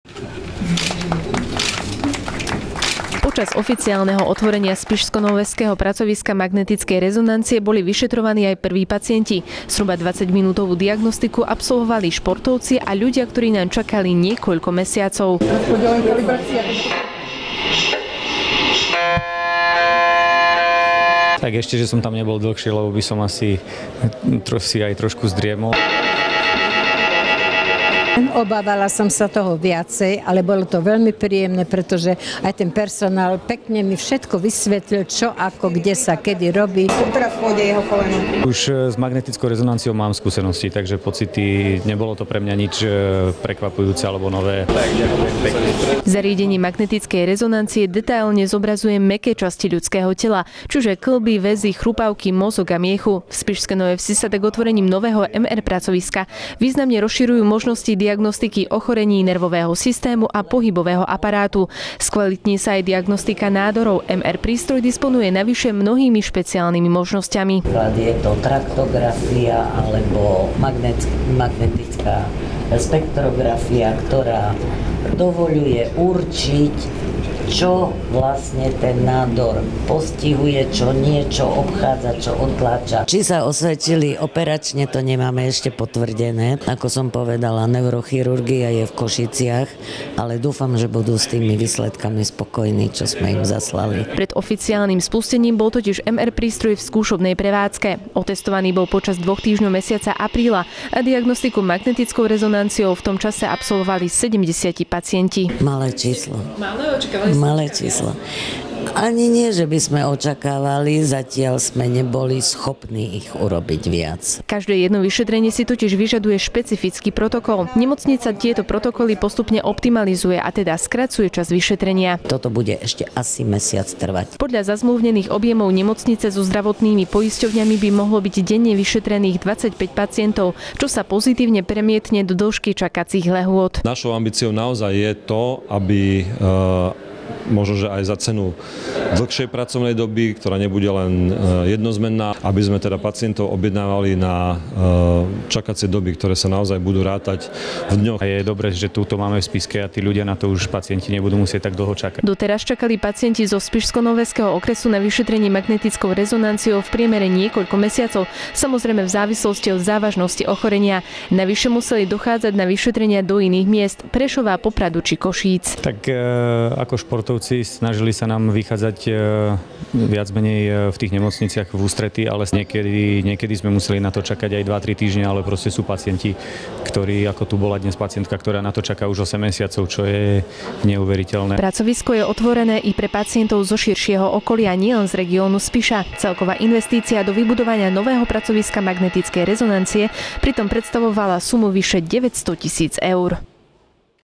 Nové pracovisko magnetickej rezonancie začalo ponúkať čakacie lehoty rátané iba na dni
Nemocnica Svet zdravia Spišská Nová Ves  oficiálne spustila nové moderné pracovisko magnetickej rezonancie (MR). Vďaka tomu sa výrazne rozširujú diagnostické možnosti nemocnice najmä v oblasti ochorení kĺbov, centrálneho nervového systému, ciev a srdca. Nové diagnostické možnosti privítajú predovšetkým pacienti s poruchami pohybového aparátu a nervového systému.